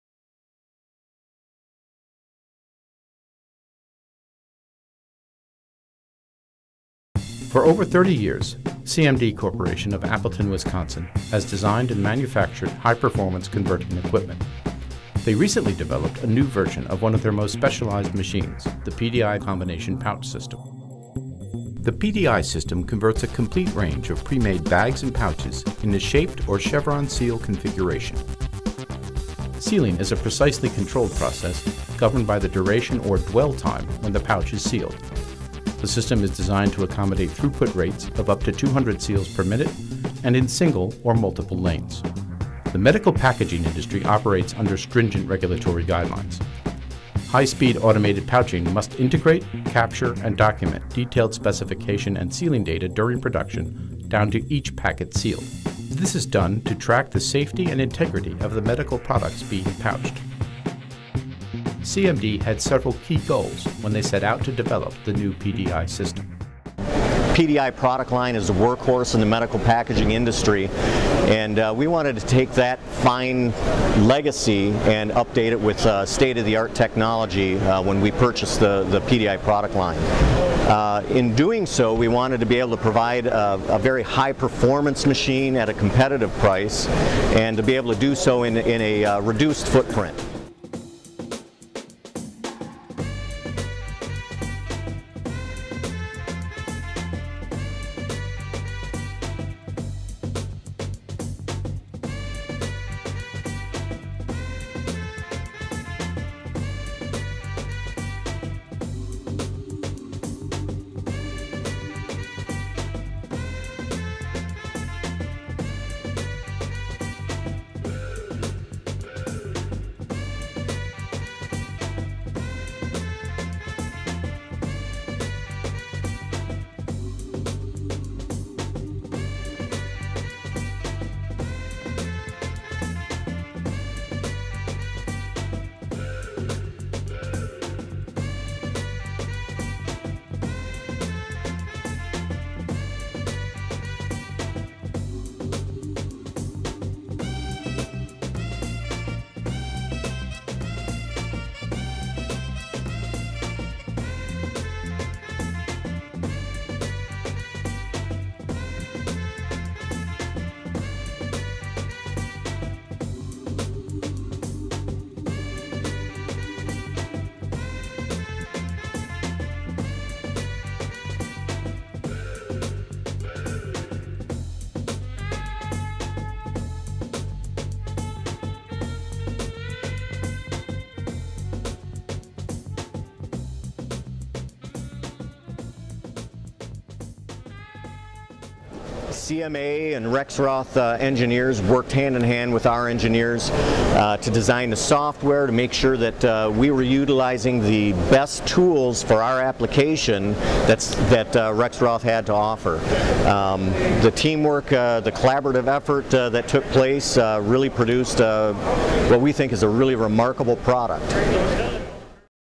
CMD Intro
This is a short video introducing one of our Wisconsin customers, CMD.  It was put together for an internal company meeting on May 22, 2013 so that everyone in the company might learn more about the companies we work with on a daily basis.